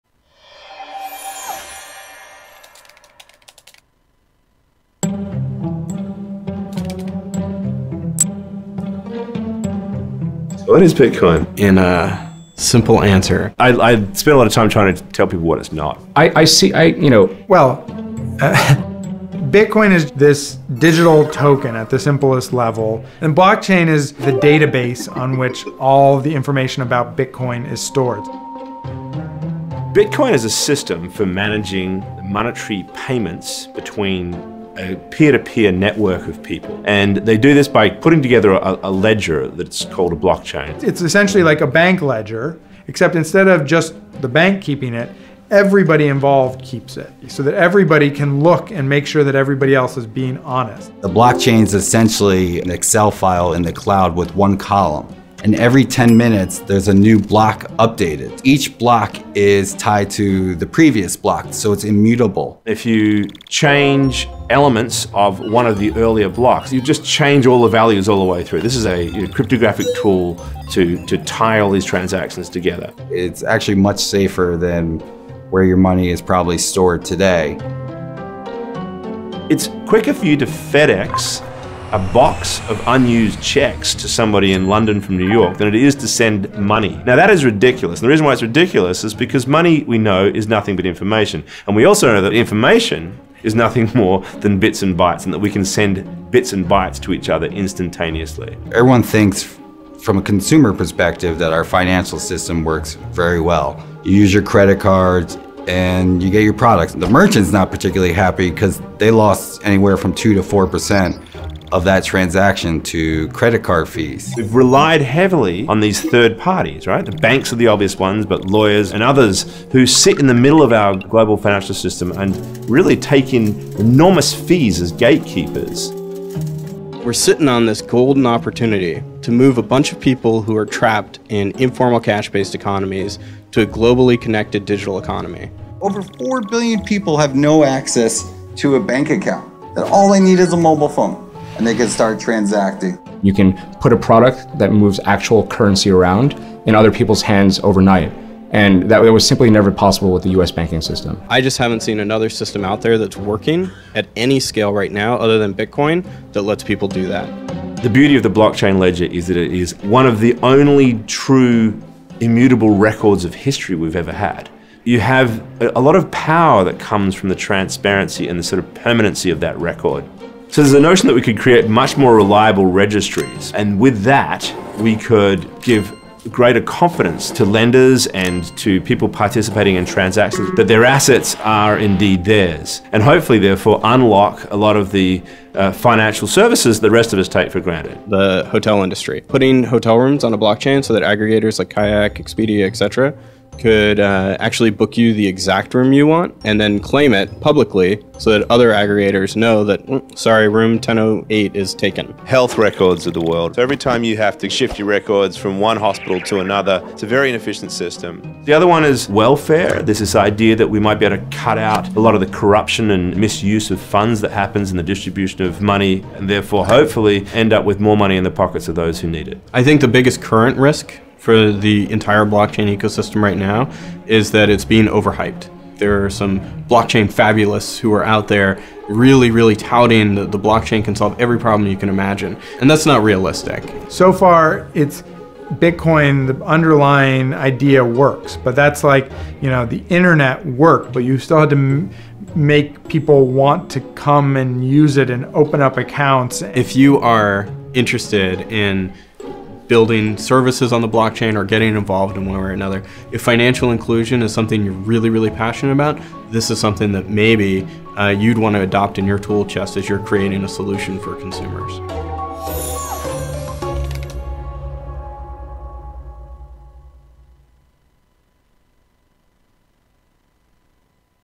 Гарвард Tech: Как блокчейн уже сейчас изменяет мир | Bitcoin Russia
Перевод субтитров к оригинальному видео Bitcoin & the Blockchain Primer от канала Osmosis Films